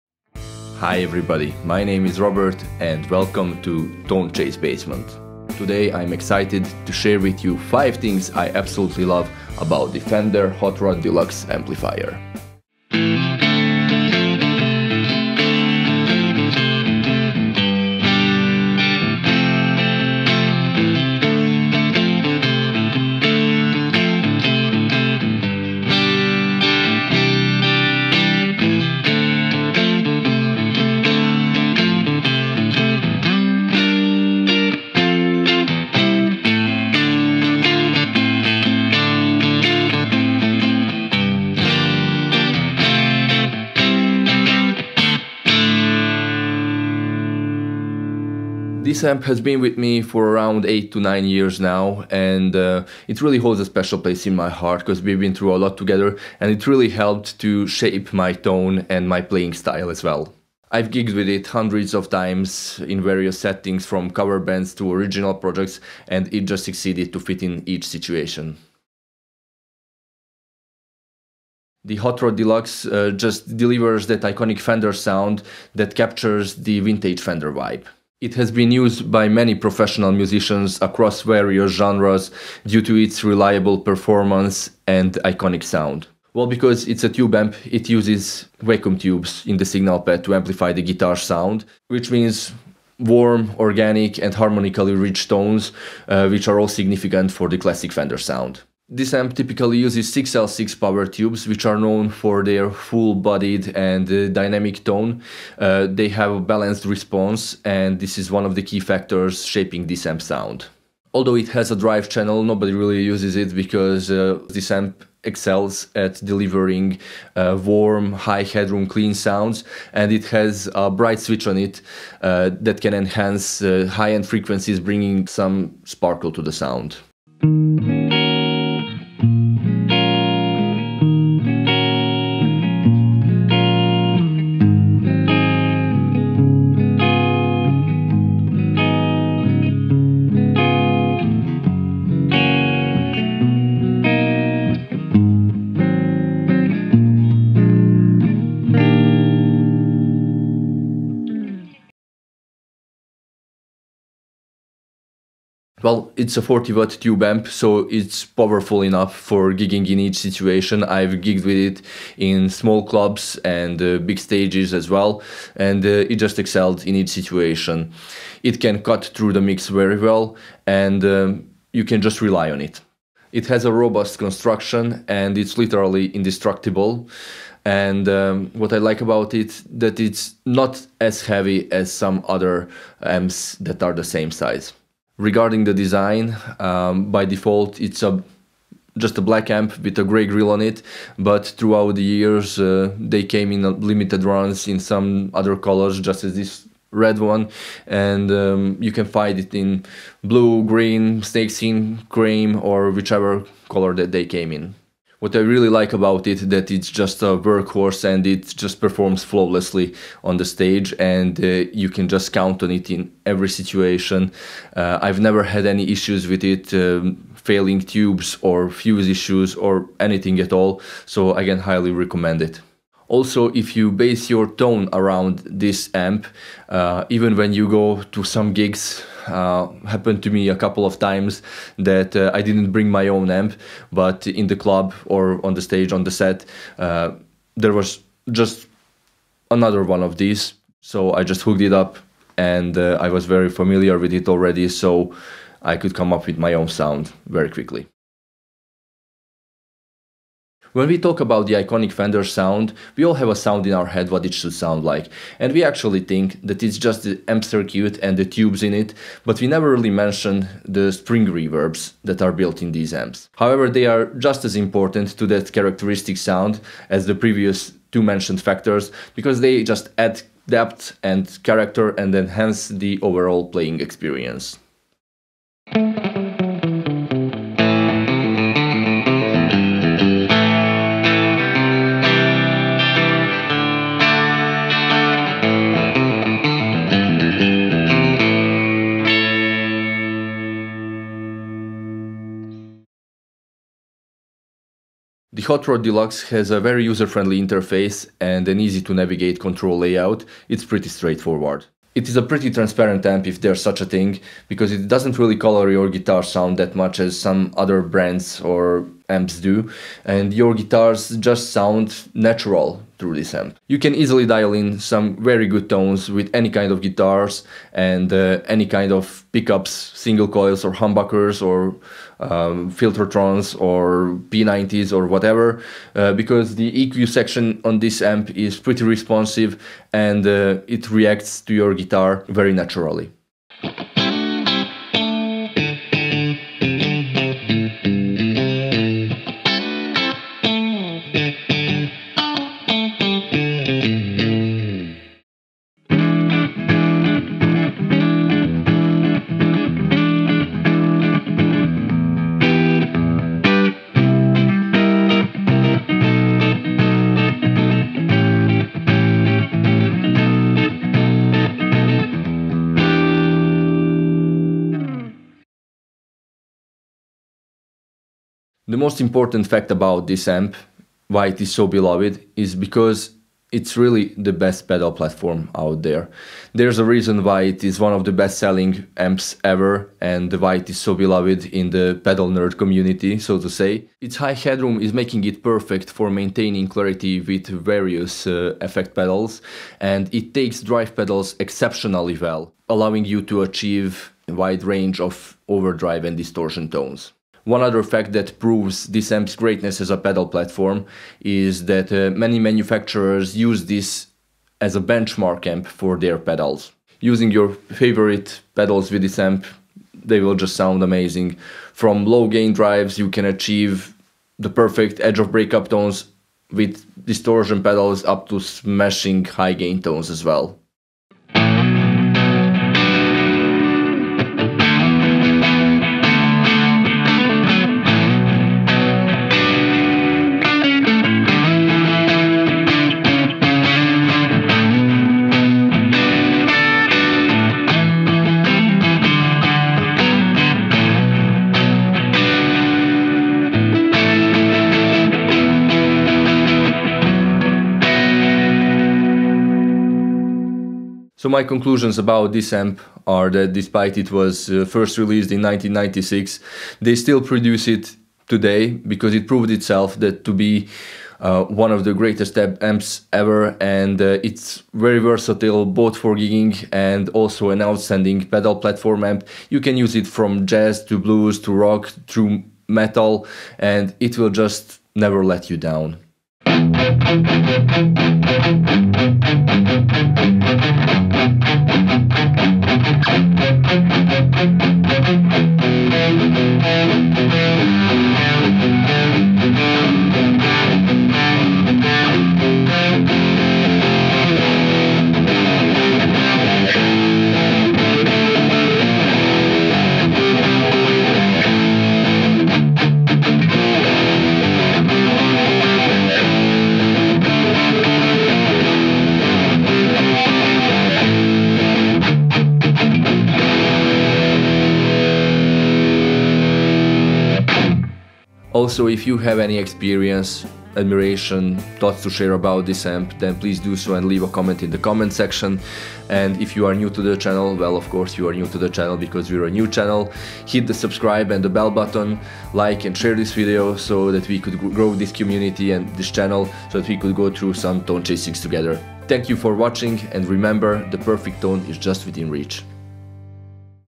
5 FACTS About The Fender Hot Rod Deluxe | The ULTIMATE Pedal Platform amp
5 FACTS About The Fender Hot Rod Deluxe | The ULTIMATE Pedal Platform amp

In this video you will find 5 facts I love about this amazing amp. In my opinion this is one of the most versatile amps out there as it is just like a cameleon and can blend into any style just perfectly with any guitar. And also the most important thing why it is in my opinion the best pedal platform amp ever.

CHAPTERS:

00:00:00 Introduction
00:00:13 Intro Jam
00:00:48 My history with the amp
00:01:09 Thing 1 - Classic Fender Tone
00:02:07 Sound Demo - Classic Fender Tone
00:02:33 Thing 2 - Gig-Worthy Durability and Timeless Design
00:04:08 Thing 3 - Built-in Spring Reverb
00:04:35 Sound Demo - Built-in Spring Reverb
00:04:51 Thing 4 - Easy to Dial In Great Tones With Any Guitar
00:05:33 Sound Demo - Tones with Strat
00:05:45 Sound Demo - Tones with Les Paul Custom
00:06:05 Thing 5 - The Ultimate Pedal Platform Amp
00:07:00 Sound Demo - Low Gain Pedal Sound with Delay
00:07:31 Conclusion
00:07:56 Sound Demo - High Gain Pedal Sound
00:08:38 Outro

Thank you for using any of these links above for supporting my channel!

Robert